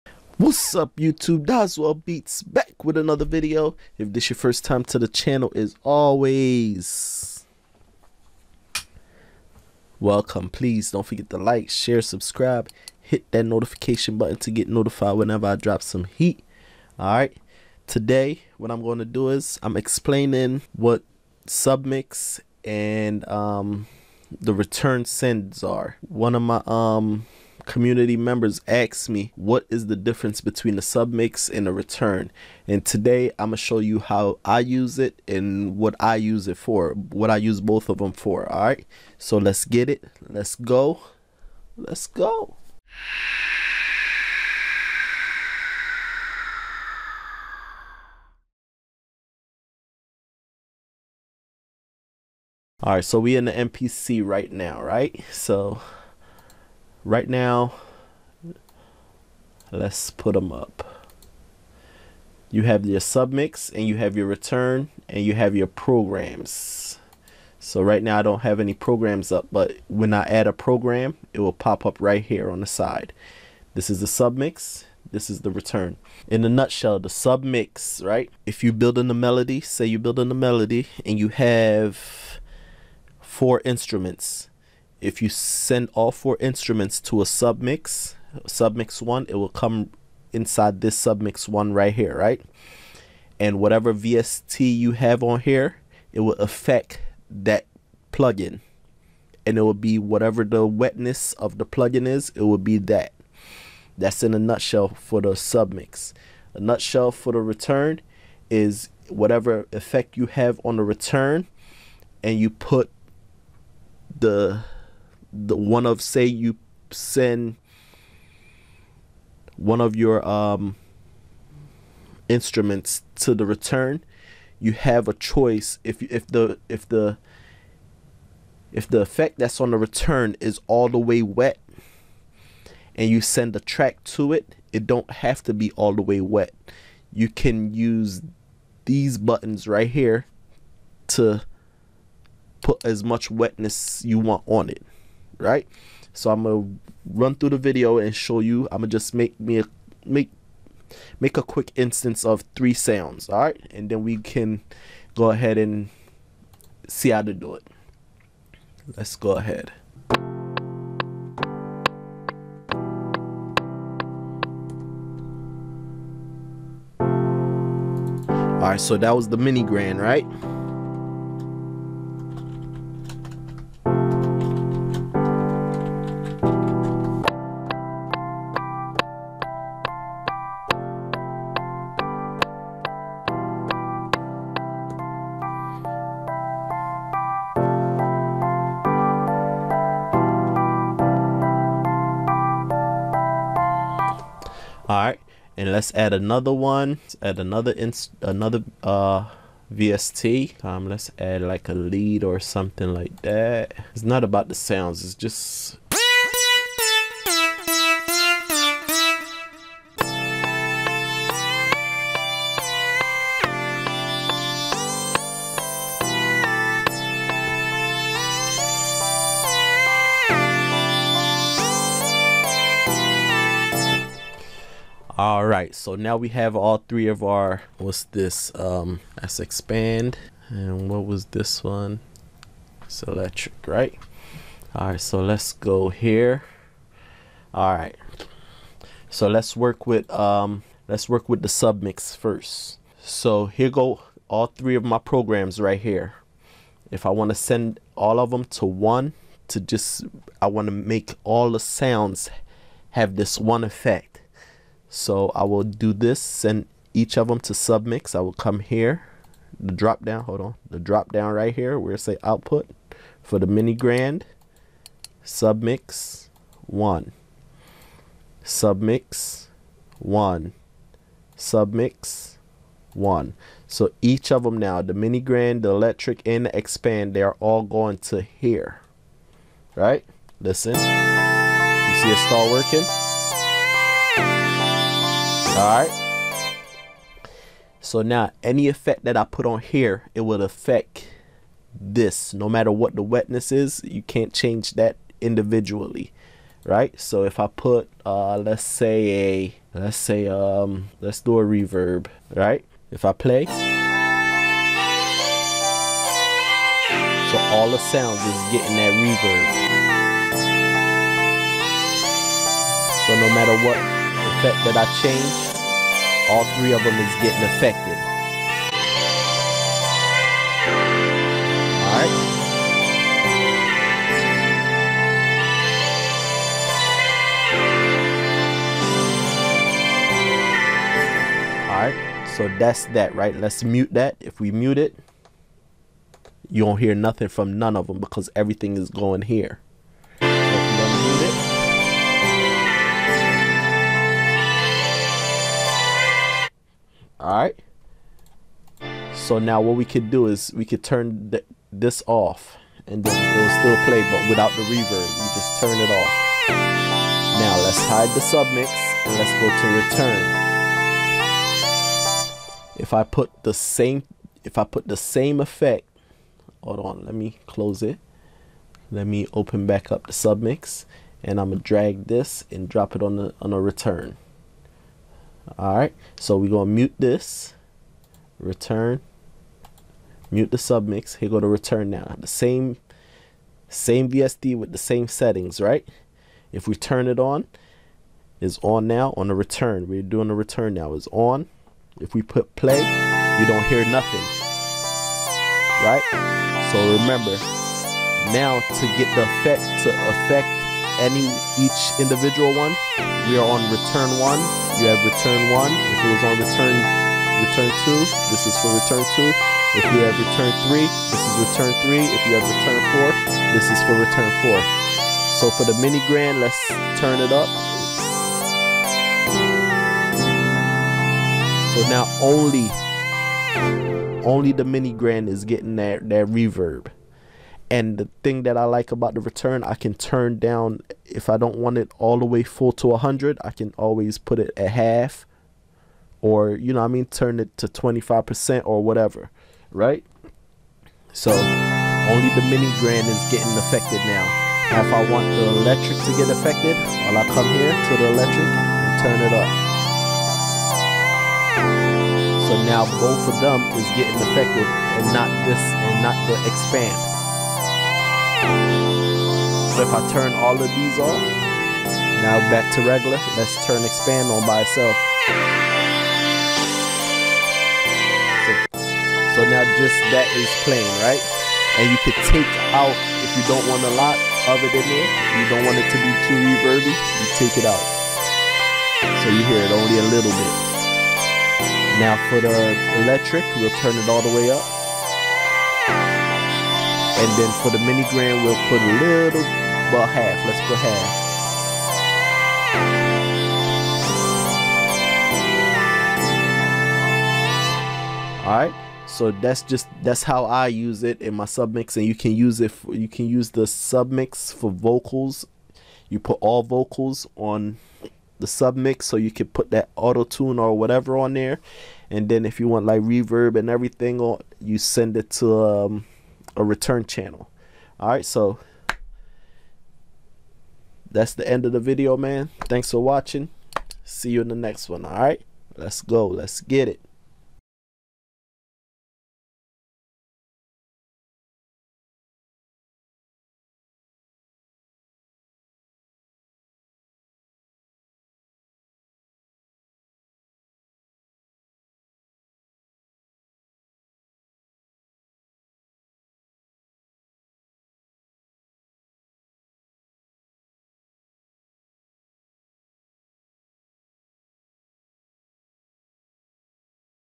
MPC Software Submix & Return how does it work Simple
mpc software submix & return how does it work simple way to use the submix and return. This video i will walk you through the way i use mpc software submix and return send in the mpc software and mpc standalone. Submix expalin and the return send explain.